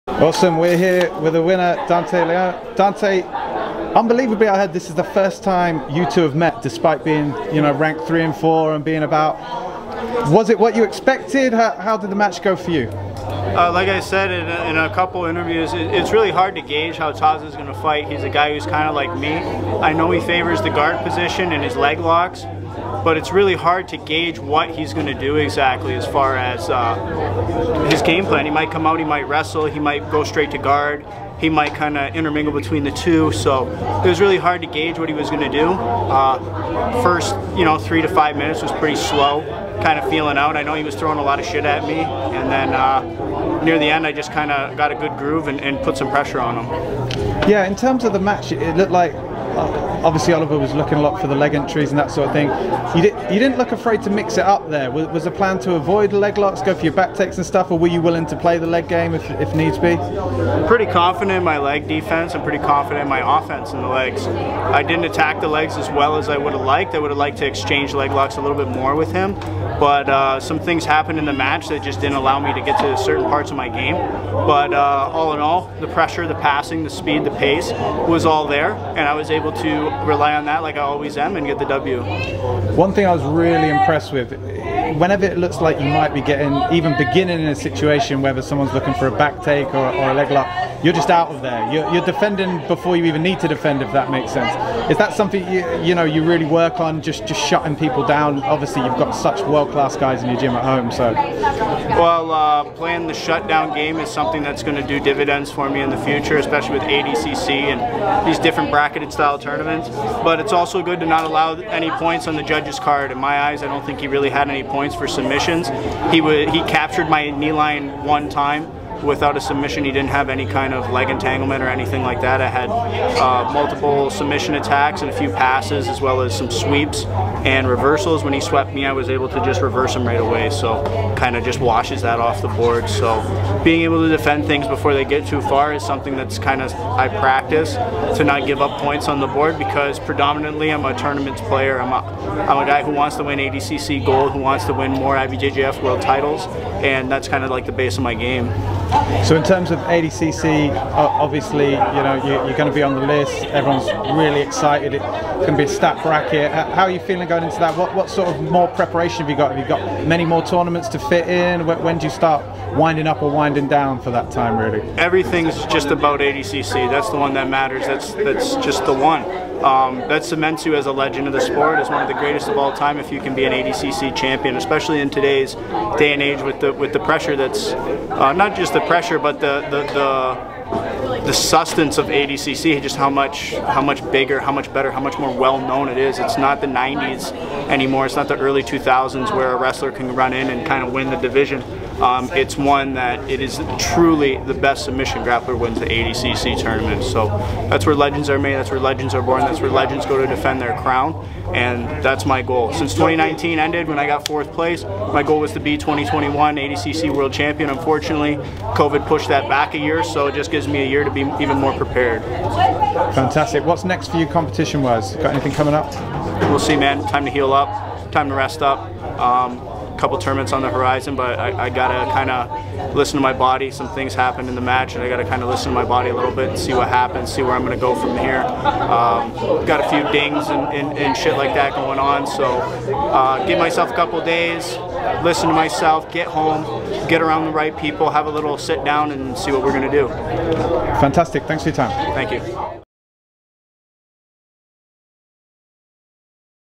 Dante Leon interview post Grapplefest 10 vs Oliver Taza
Dante Leon discusses his unanimous decision victory over Oliver Taza, his ADCC prep and what the future holds.